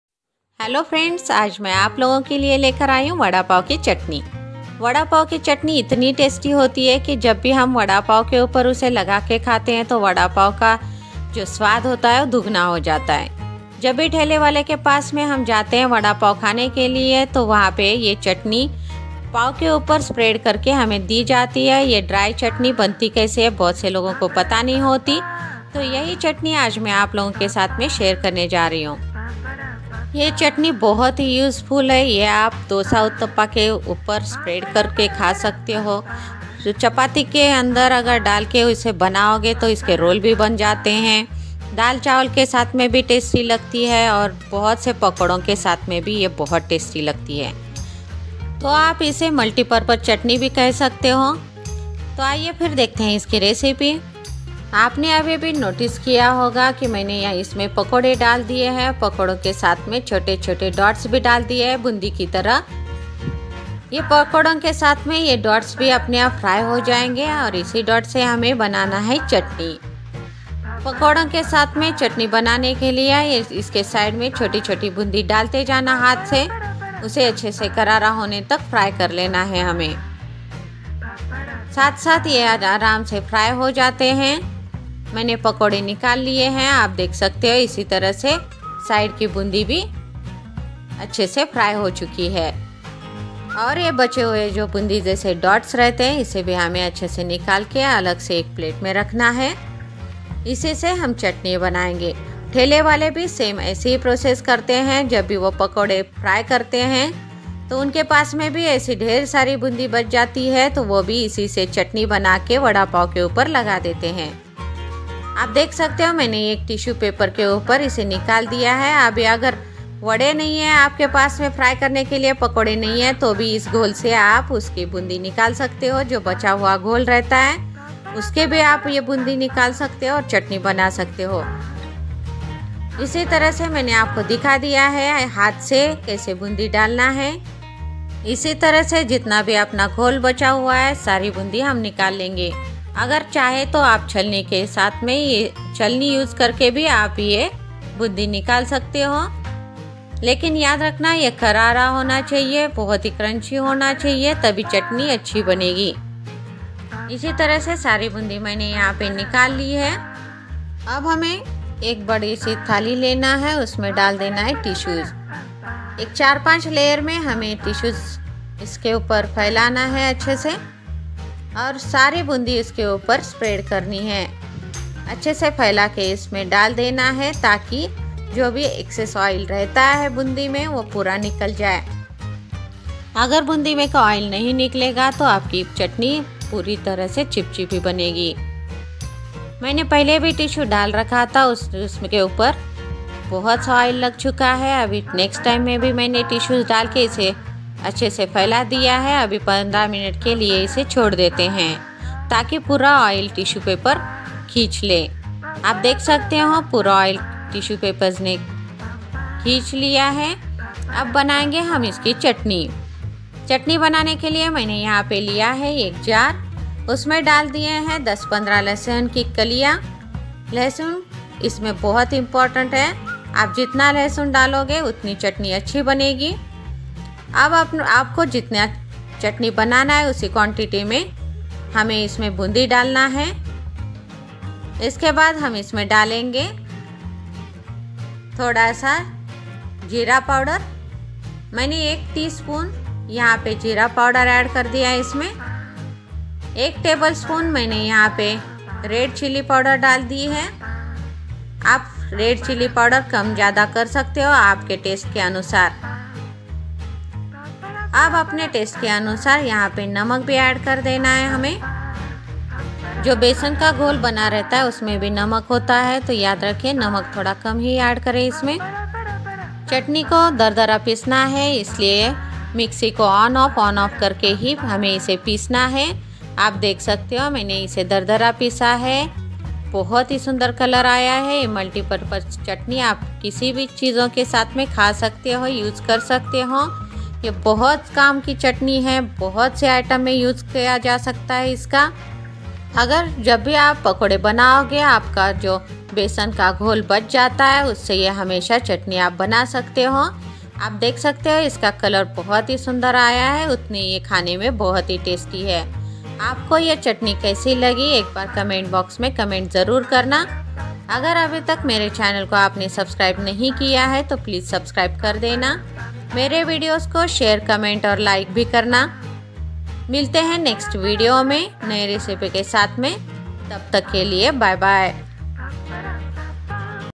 ठेले वाले जैसी ऐसी बनती है वडापाव की चटनी।वडापाव चटनी रेसिपी।वडापाव की सूखी चटनी रेसिपी।
Hello foodies,
Friends aaj main aap logon ke liye aur ek nai dish lekar aai hun.Aur is dish ka nam hai "Vadapav Ki Chatni"Jab bhi ham thele wale ke pass mein jaate hain vada pav khane ke liye,to vada pav ke andar ek chatni sukhi chatni hamen di jaati hai usmein bharkar vahi chatni aaj maine yahan per sikhaya hai.Hum logon ko vah chatni bahut hi testi lagti hai.Ye multipurpose chatni hai.Yah aap dosa, uthappa ke upar spread kar sakte ho,iske chapati roll bana sakte ho,daal chawal ke sath mein bhi kha sakte ho,sare pakado ke sath mein bhi enjoy kar sakte ho.Es tarah bahut se chijon ke sath mein aap ise enjoy kar sakte ho.Isiliye yah multipurpose chatni bahut kam ki hai.Aap ise jarur ek bar try karna. Aur ye chatni aapko kaise lagi comment box mein jarur comment karna .
chutney ek uses anek.
Thank u.